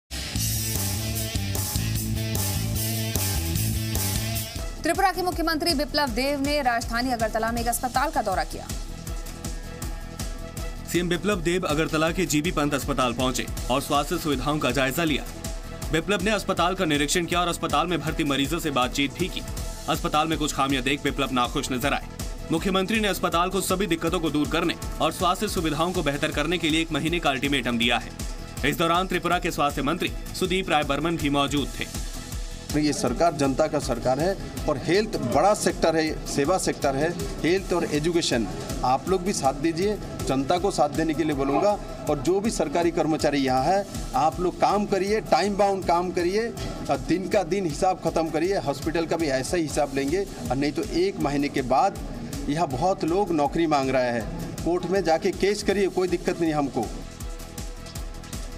Breaking 20-20: Tripura CM inspects a hospital in Agartala
Tripura CM Biplab Deb inspected Agartala's GB Pant hospital. He examined things thoroughly & talked to the patients too. He has asked the concerned authorities to improve the health facilities. Watch this video to know more.